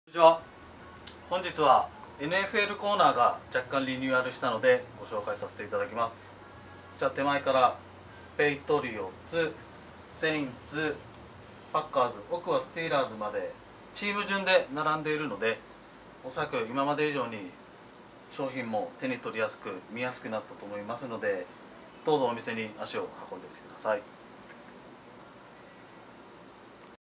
NFLコーナーがリニューアルしました!!
好きなチームがなかなか見つからない!ほしいアイテムがない。
そんな皆様の声を解消すべきNFLコーナーチーム順に並び替えました!
是非店頭でご確認下さい。